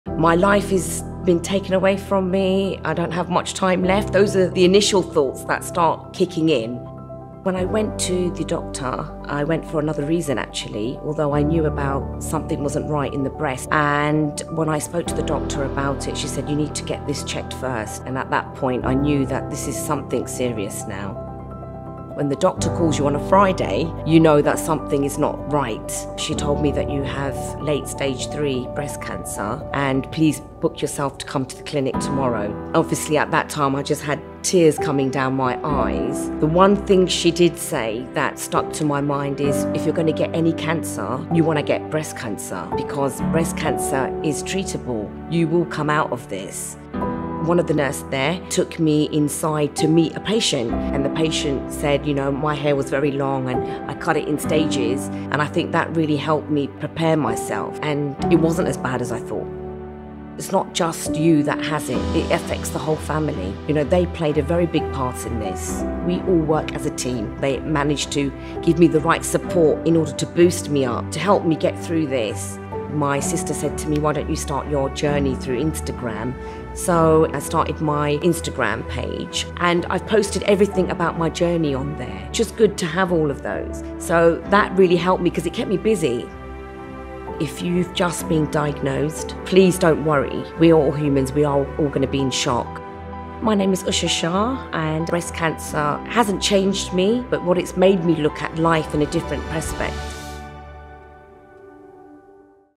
Breast Cancer Patient Usha Shah's Story - Al Shunnar Plastic Surgery
Let’s live with a unified purpose to fight breast cancer.
Beautiful Usha defeated breast cancer and here is her inspiring story.

لنعش من أجل هدف موحد بمحاربة سرطان الثدي.
أوشا الجميلة تغلبت على سرطان الثدي، إليكم قصتها الملهمة.

Villa 591 Jumeirah Beach Road Dubai, UAE